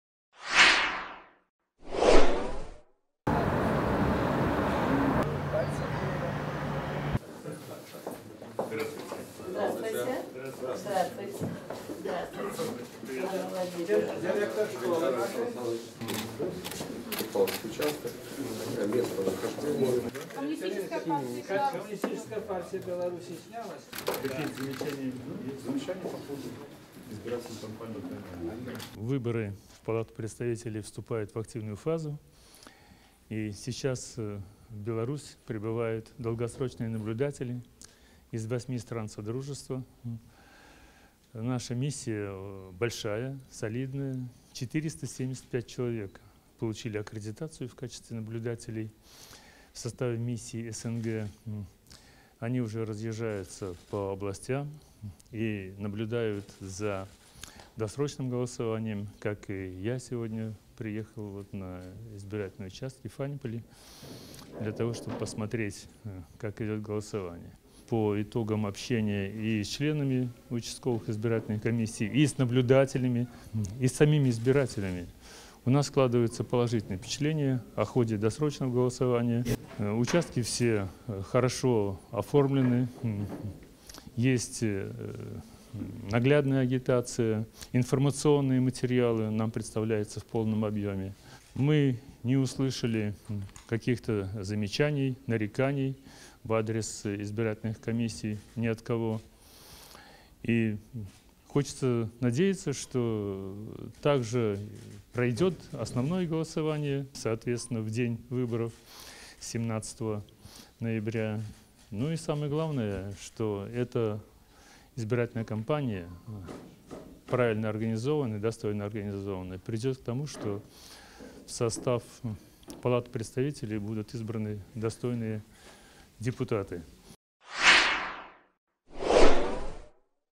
Лебедев: у миссии СНГ положительное впечатление о ходе досрочного голосования в Беларуси
У миссии наблюдателей от СНГ складывается положительное впечатление о ходе досрочного голосования на выборах в Палату представителей седьмого созыва. Об этом журналистам сообщил глава миссии Сергей Лебедев.
В Беларуси находятся долгосрочные наблюдатели из восьми стран СНГ. Всего в составе миссии Содружества 475 человек, которые наблюдают за досрочным голосованием в разных регионах страны.